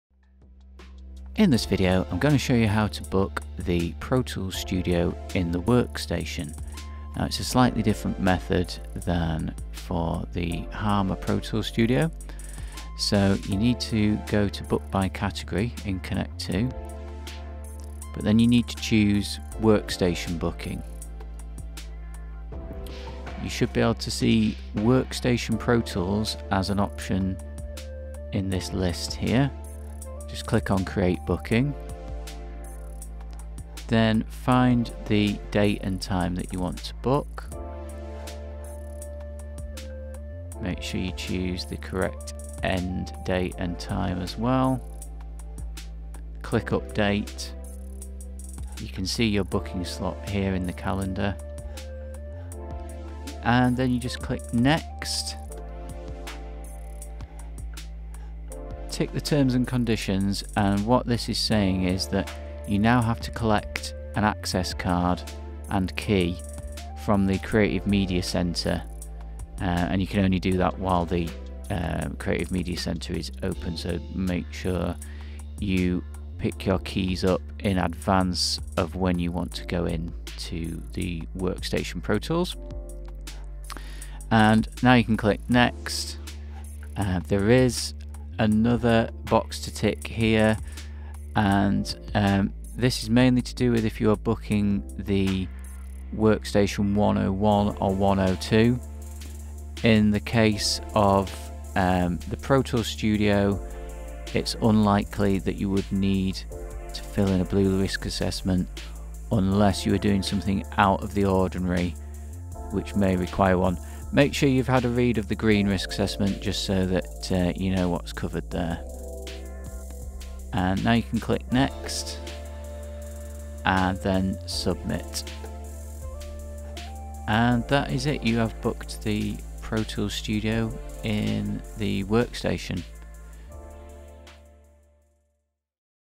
Connect2: How to book the Workstation Pro tools studio
How to book the Workstation Pro tools studio using the Connect2 booking system.  For students at Sheffield Hallam University studying Film & TV Production.
Please note you must have completed an induction in order to book the studio.